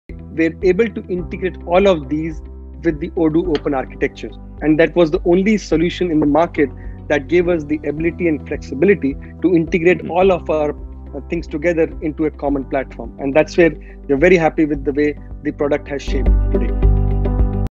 Client Testimonial | Anand Choudha, Founder, AmiViz | Brainvire Infotech
The video shows the positive feedback of one of Brainvire’s clients. Anand Choudha, Founder, AmiViz, shared the impact of Odoo implementation on their business and how they observed a peaked interest of sellers, vendors, and others on the Odoo mobile application. This resulted in a considerable jump in the industry. Anand was glad and optimistic as all these changes made by Brainvire helped them understand customers better and strengthen their bond.

About Brainvire